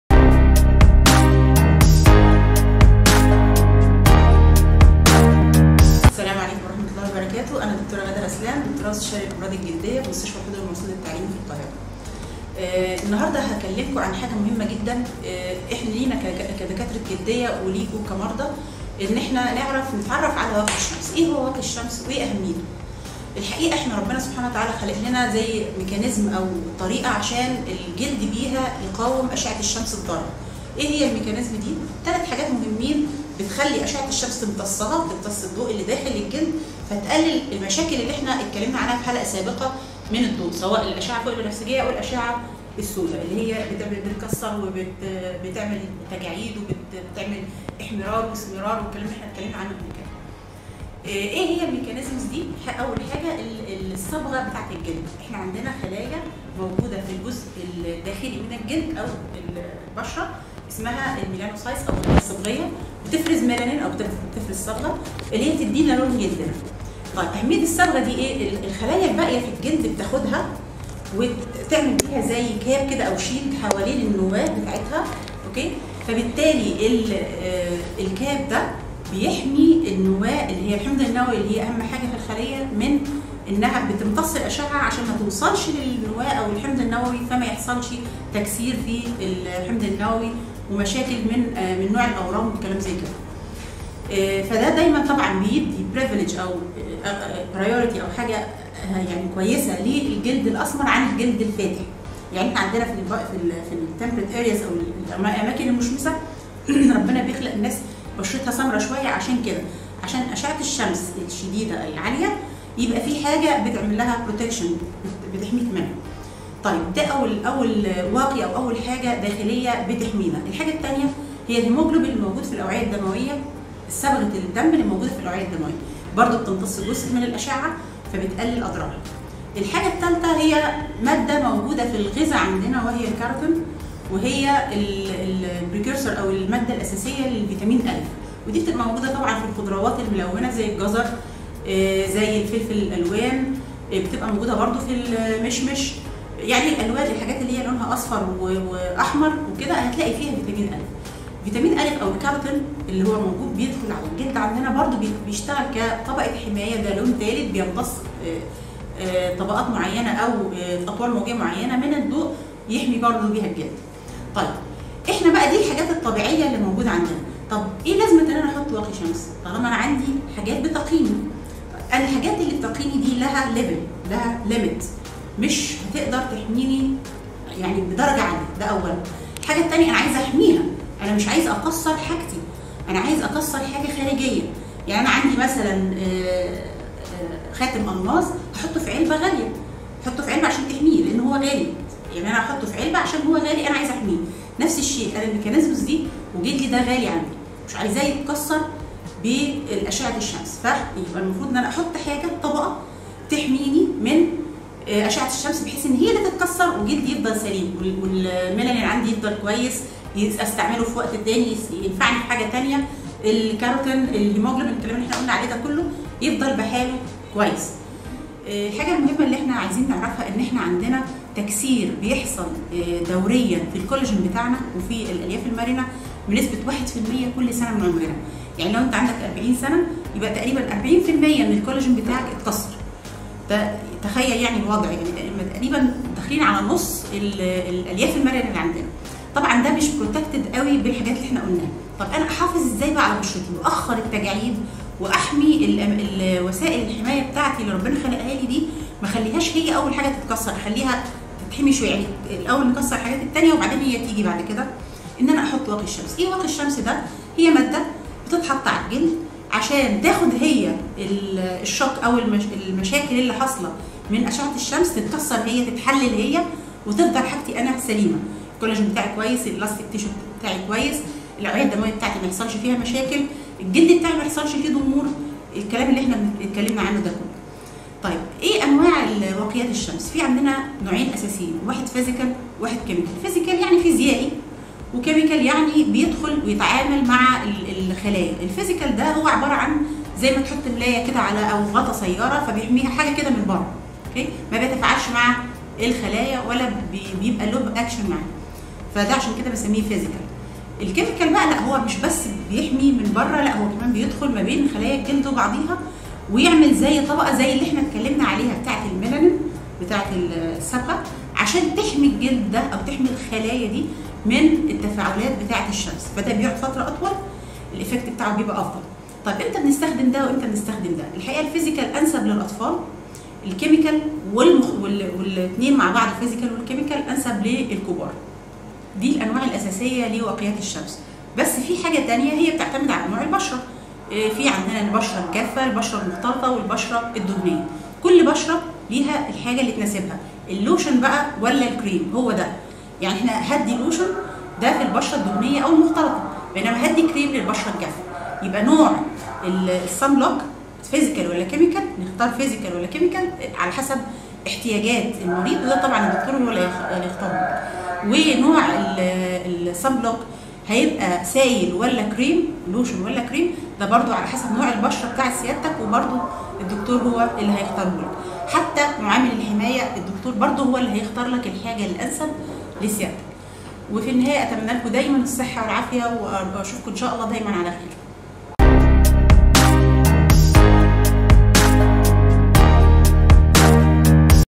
قناة تعليمية للاستاذة الدكتورة غادة رسلان تخص الجلدية والليزر والرشاقة والتجميل
"بما إننا فى الصيف والجو صعب، والشمس بتكون قوية جداً 🥵☀
وطبعاً بيكون ليها أضرار على البشرة ❌

حبيت نتكلم عن (( واقى الشمس )) 🎞👇
إيه هو واقى الشمس ؟
وإيه فكرته ؟!
وقد إيه مهم جداً وبيحمى البشرة من أشعة الشمس الضارة ؟!!